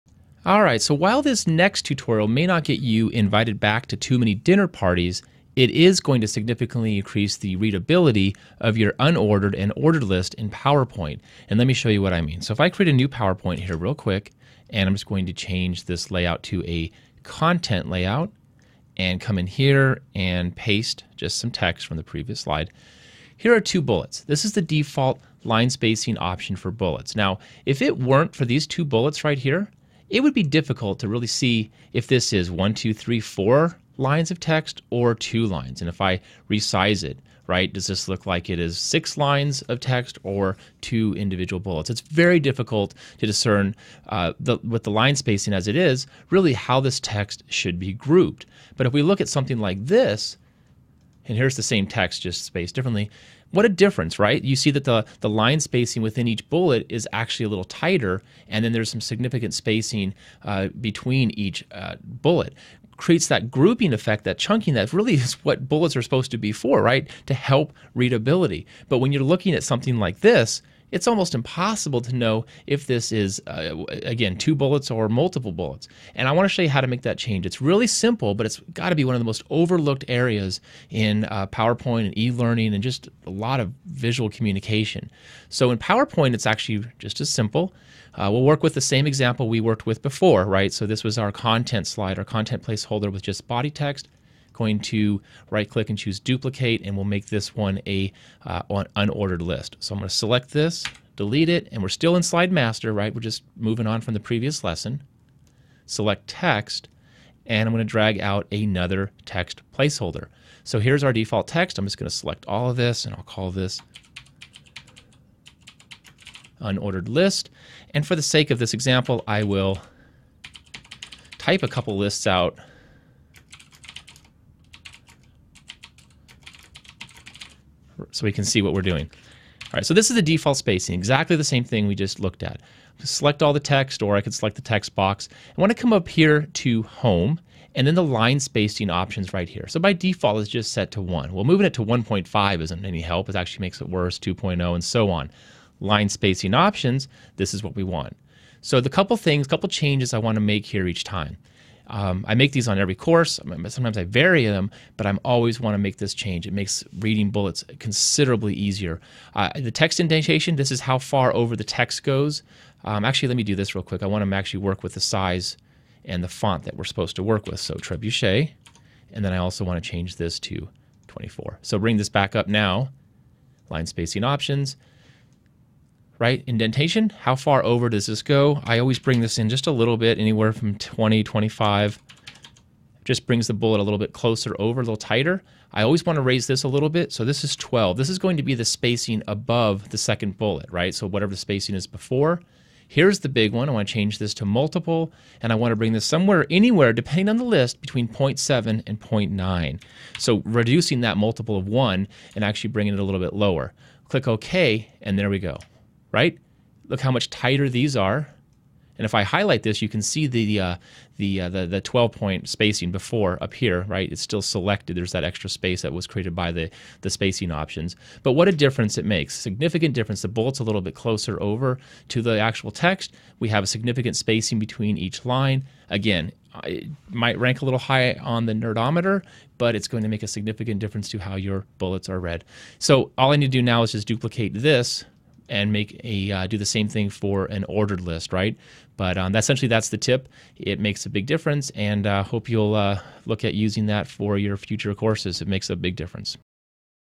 PowerPoint Tutorial: How to increase readability of bullet points
Here's how to increase the readability of bullets & lists in PowerPoint